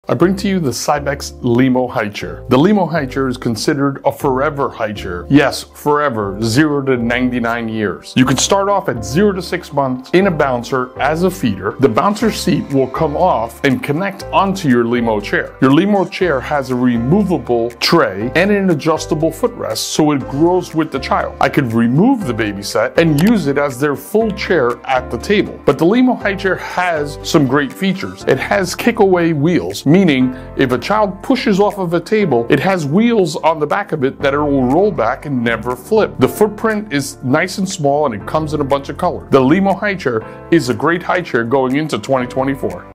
Cybex Lemo High Chair 👶furniture
A forever high chair 🤩 The CybexLemoHighChair can be used from birth to 99 years! With different attachments that caters every stage of life, this is a chair you’d want to grow with your baby!
Leave your questions in the comments 💬